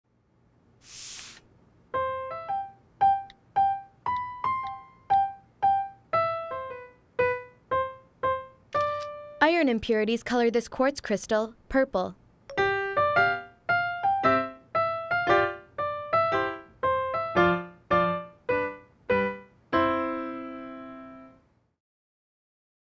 Tap. Snap. Speak. Make flashcards with the StudyBlue Android app.
Make online flashcards with text, images and voice using the free StudyBlue mobile flashcards Android app.